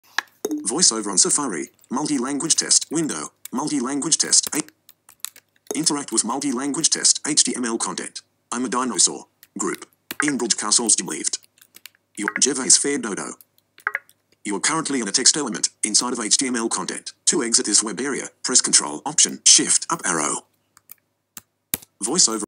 Multi language test OS X 10.11.6
VoiceOver on OS X 10.11.6 combined with Safari 9.1.2 doesn't automatically switch language module.